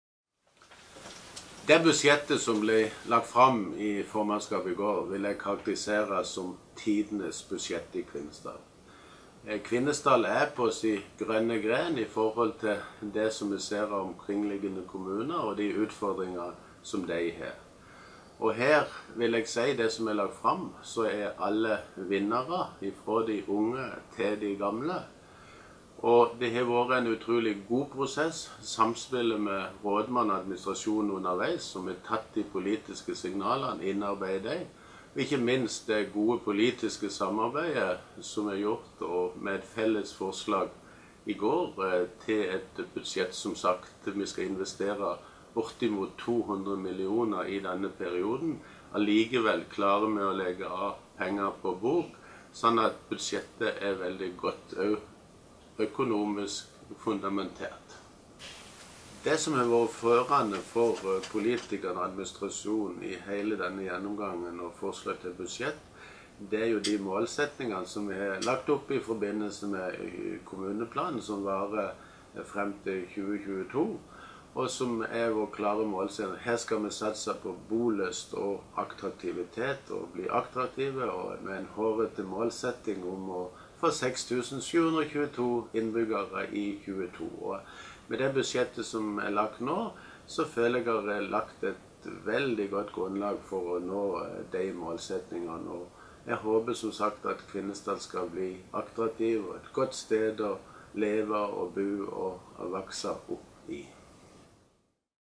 Hvorfor gjør kommunen tidenes satsing?
Video med innledning til hvorfor kommunen har vedtatt tidenes budsjett. - Kvinesdal er på den "grønne gren" i forhold til omkring liggende kommuner. Av det som legges fram i økonomiplanen er alle vinnere. Fra unge til gamle. Det har vært en utrolig god prosess mellom rådmann og administrasjon som har tatt de politiske signaler underveis og ikke minst de politiske samarbeidet, sier ordføreren.